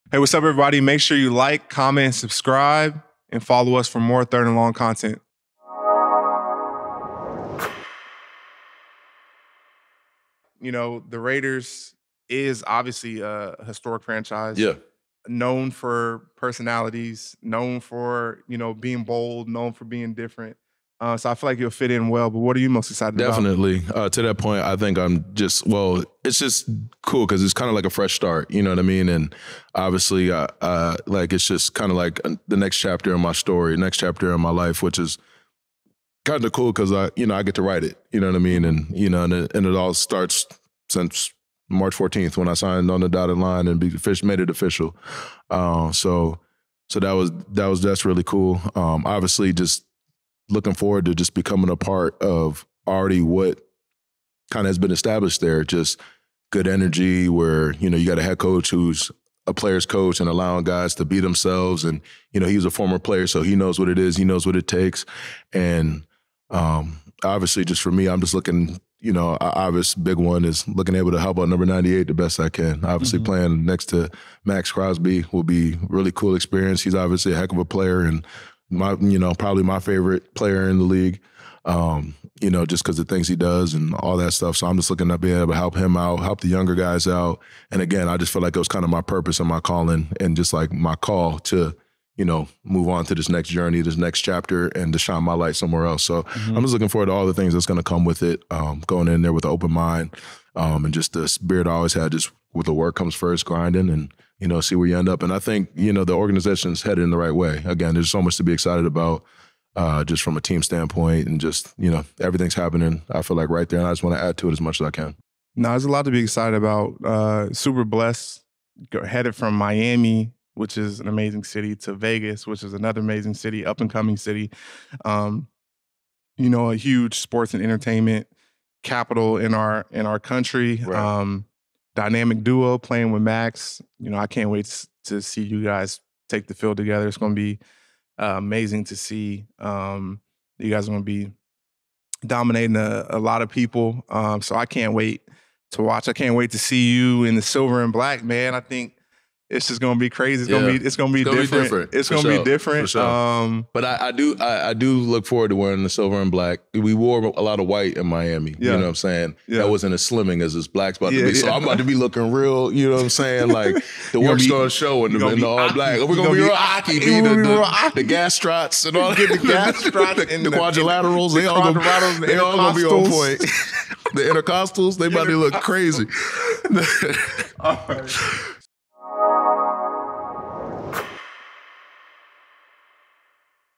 Christian Wilkins on why he chose the Raiders and why he's excited for their upcoming season
Follow us on Arik and Tidal League on socials